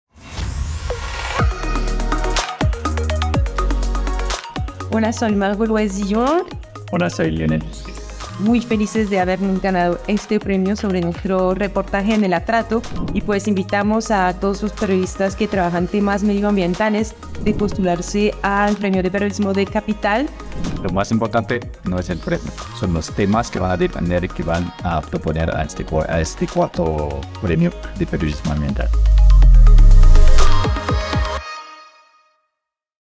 Premio al periodismo Social y Ambiental
🎤🌎 ¡Atención periodistas y futuros comunicadores! Constructora Capital te invita a participar en su Premio de Periodismo Social y Ambiental. Si has trabajado en temas de conservación, desarrollo comunitario o innovación social, ¡Esta es tu oportunidad para brillar! ¡Inscríbete ahora!